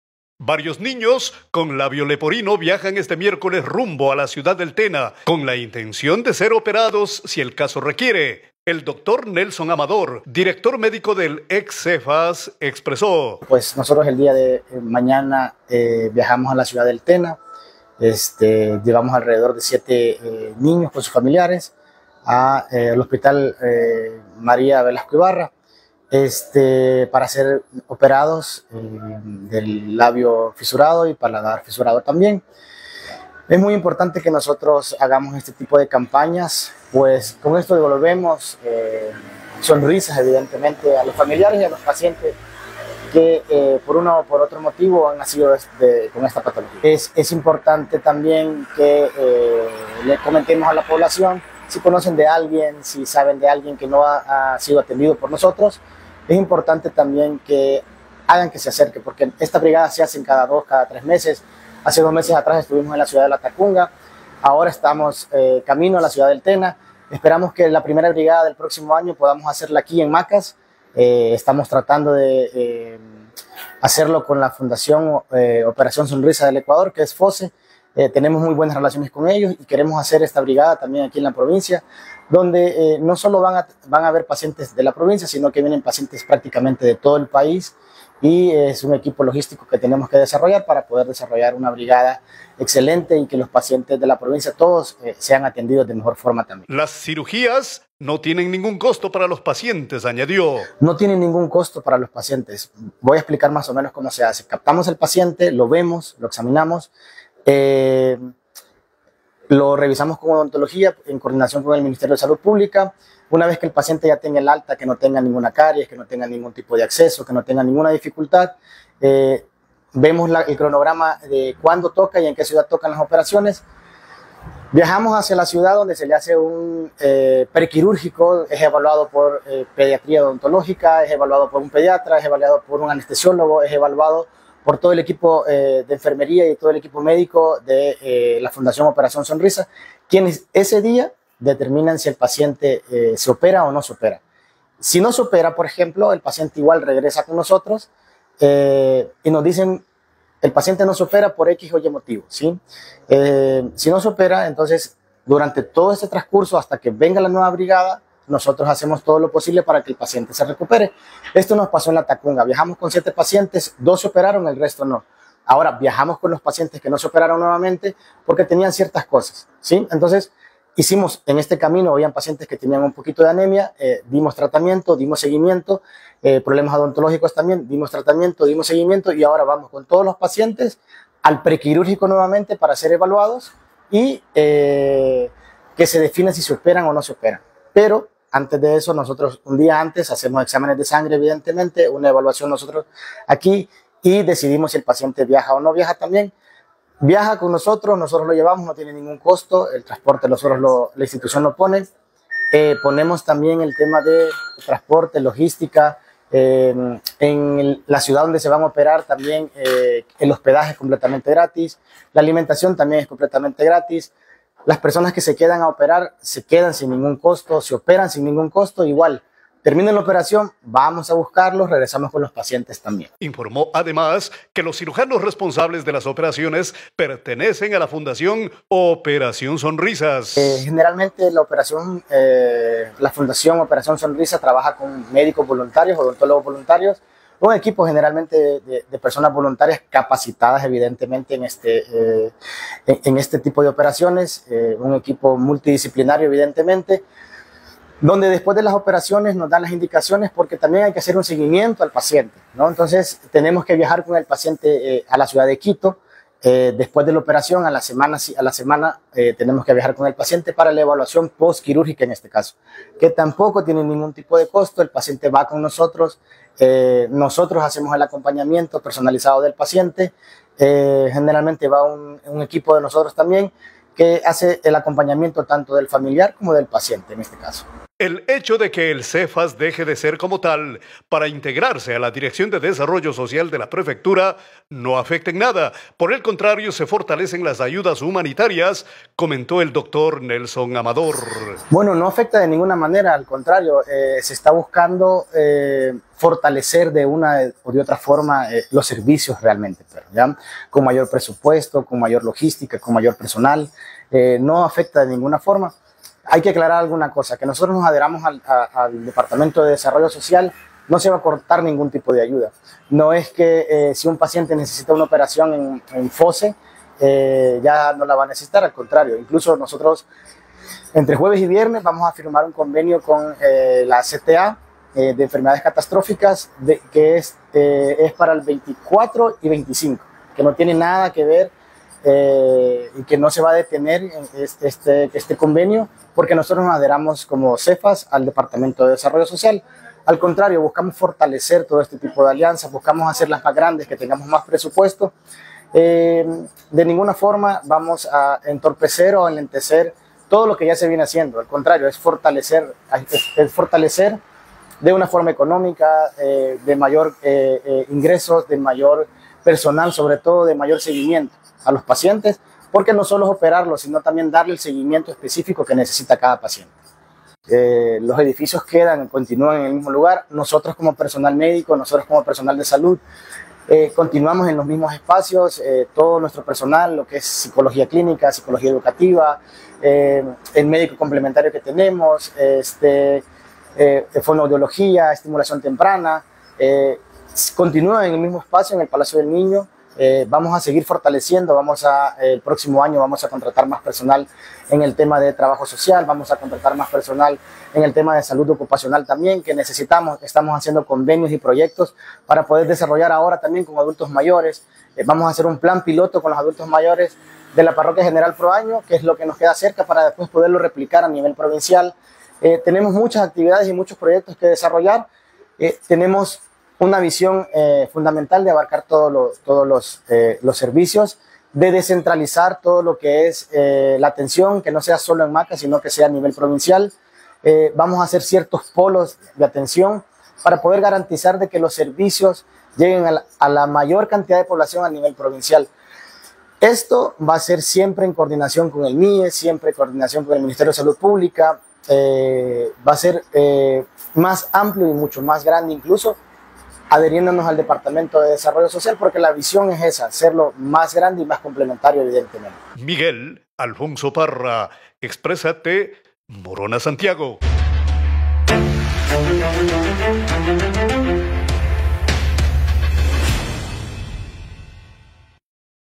Menores pueden ser registrados en Morona Santiago para "Operación Sonrisas"
[SOCIAL] IMPORTANTE: MoronaSantiago, varios niños con labio leporino, viajan este miércoles 8 de noviembre a la ciudad del Tena con la esperanza de ser evaluados para ingresar a cirugía correctiva asumida por la fundación "Operación Sonrisas".

El Dr. Nelson Amador, director médico del ex Cefas, informó que el paciente y sus familiares no tienen que asumir ningun costo. Invitando a padres de otros niños con la misma condición a que se inscriban para recibir este beneficio.